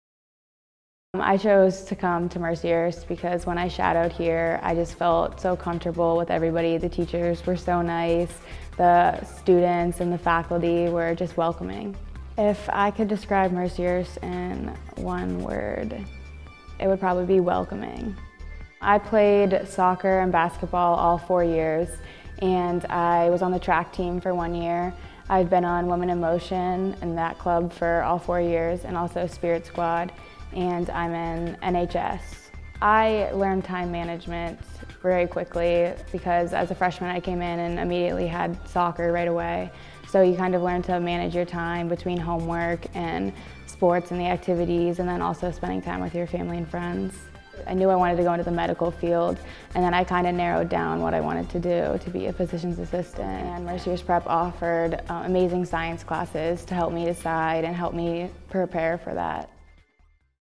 Lindsey Friello: MPS Class of 2017
Time management skills helped Lindsey Friello balance academics, numerous sports and clubs to make her high school years happy and successful.  She is a 2017 Mercyhurst Prep graduate.